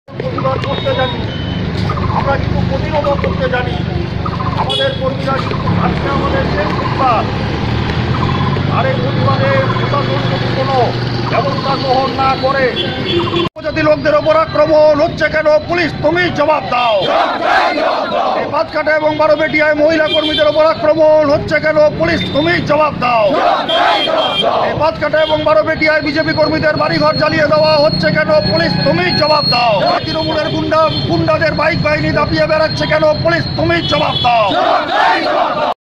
bjp thana gherao থানা ঘেরাও কর্মসূচি
পঞ্চায়েত নির্বাচনে বিজেপি প্রার্থীদের প্রাণনাশের হুমকি, মারধর ও সন্ত্রাস চালাচ্ছে শাসক দল তৃনমূল বলে অভিযোগ জেলা বিজেপি'র। মনোনয়ন প্রত্যাহার করতে হবে এই কারনে জেলা জুড়ে সন্ত্রাস চালাচ্ছে তৃনমূল। ঘটনার প্রতিবাদ জানিয়ে ও বিজেপি কর্মীদের নিরাপত্তা চেয়ে সোমবার দুপুরে বিজেপি'র জলপাইগুড়ি জেলা সভাপতি দেবাশীষ চক্রবর্তীর নেতৃত্বে চললো থানা ঘেরাও কর্মসূচি। full story